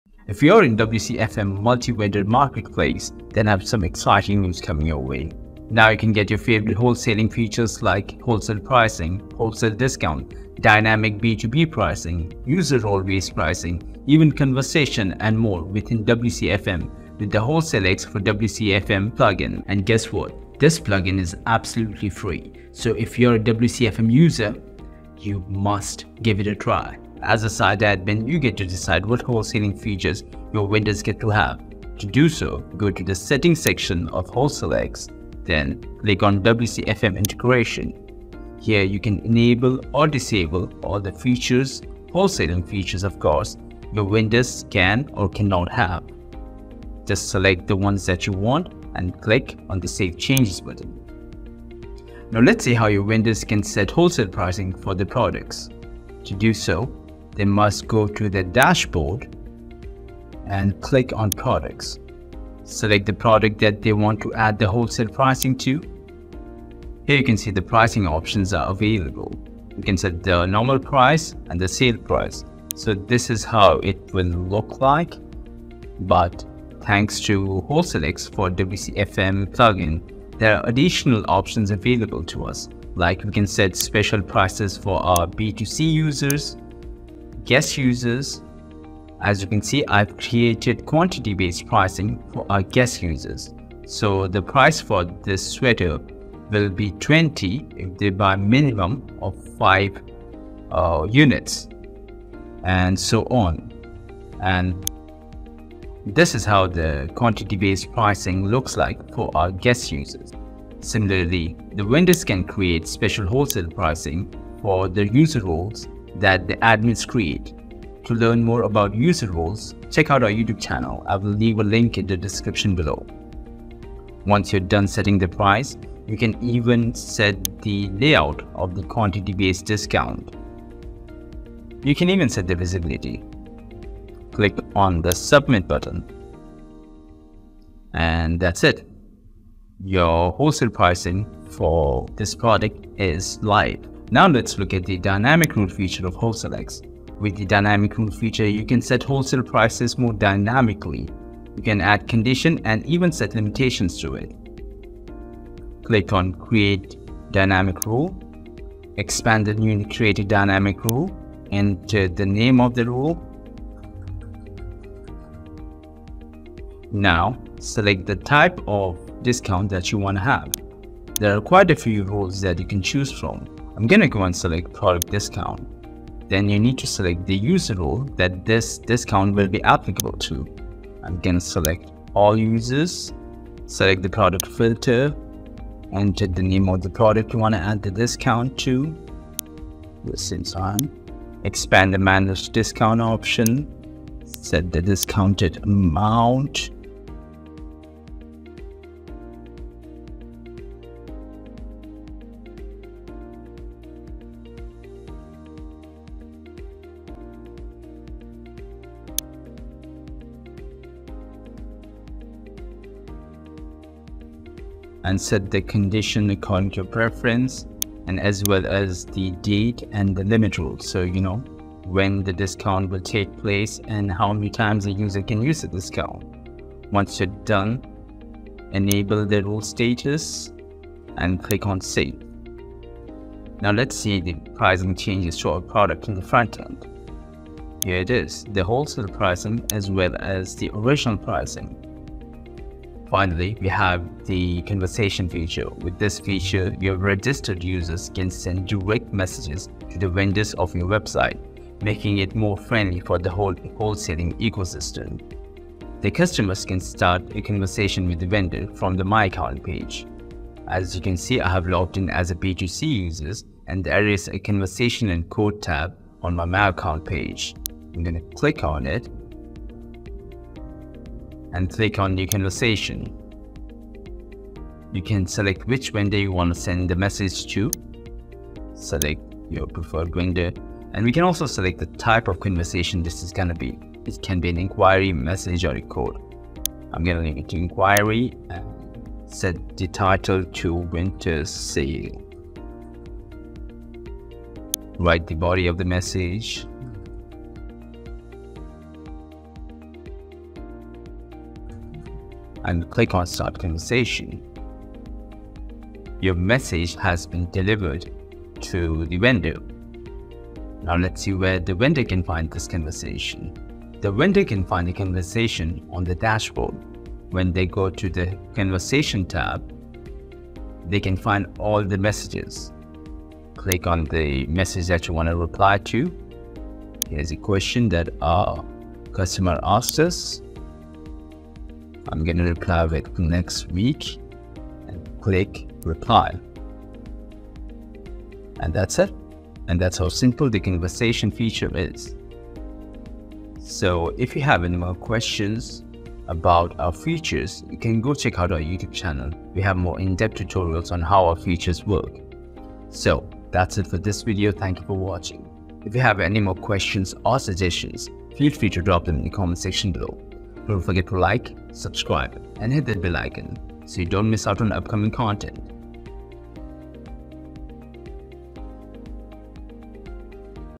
How to Add Wholesale Features in WCFM Multivendor Marketplace
Do you want wholesaling features in your WCFM multivendor marketplace? We have the perfect solution!

Are you ready to take your WCFM multivendor marketplace to the next level? Unlock the power of B2B wholesaling with WholesaleX! Seamlessly integrating with WooCommerce and WCFM, WholesaleX empowers you to transform your ordinary marketplace into a thriving B2B multivendor powerhouse.

👉From the video, you learn to:

✅User role-based wholesale prices
✅Dynamic Rules
✅Built-in conversation system

⏰Chapters
00:00 Introduction
00:26 Settings
00:56 Wholesale Pricing
02:23 Dynamic Wholesale Pricing
03:53 Conversion Features